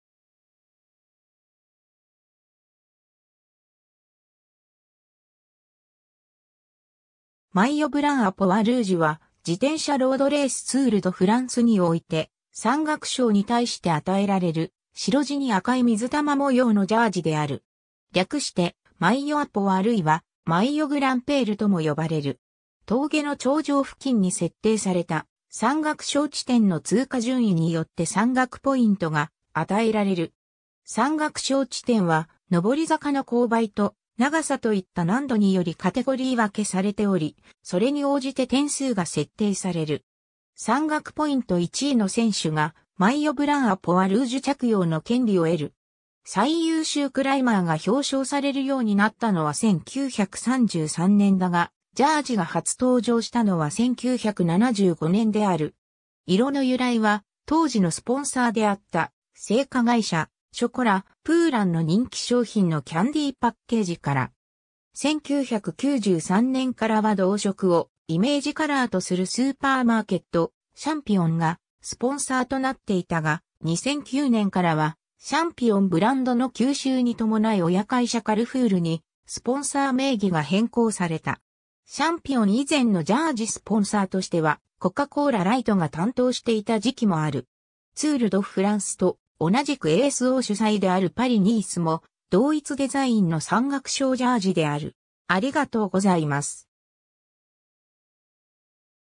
マイヨ・ブラン・ア・ポワ・ルージュ
ツール・ド・フランス
マイヨ・ブラン・ア・ポワ・ルージュ (maillot blanc à pois rouges) は、自転車ロードレース・ツール・ド・フランスにおいて、「山岳賞」に対して与えられる、“白地に赤い水玉模様”のジャージである。
略して「マイヨ・ア・ポワ（・ルージュ）」あるいは「マイヨ・グランペール (maillot grimpeur)」とも呼ばれる(実際の発音は「マイヨ・グランプール」が近い)。
 峠の頂上付近に設定された山岳賞地点の通過順位によって山岳ポイントが与えられる。
山岳賞地点は、登り坂の勾配と長さといった難度によりカテゴリー分けされており、それに応じて点数が設定される（ツール・ド・フランス 山岳ポイント表参照）。
山岳ポイント1位の選手がマイヨ・ブラン・ア・ポワ・ルージュ着用の権利を得る。
 最優秀クライマーが表彰されるようになったのは1933年だが、ジャージが初登場したのは1975年である。
色の由来は当時のスポンサーであった製菓会社「ショコラ・プーラン」の人気商品のキャンディーパッケージから。
1993年からは同色をイメージカラーとするスーパーマーケット「シャンピオン」がスポンサーとなっていたが、2009年からは「シャンピオン」ブランドの吸収に伴い親会社カルフールにスポンサー名義が変更された。
「シャンピオン」以前のジャージスポンサーとしてはコカ・コーラ・ライトが担当していた時期もある。
 ツール・ド・フランスと同じくASO主催であるパリ〜ニースも、同一デザインの山岳賞ジャージである。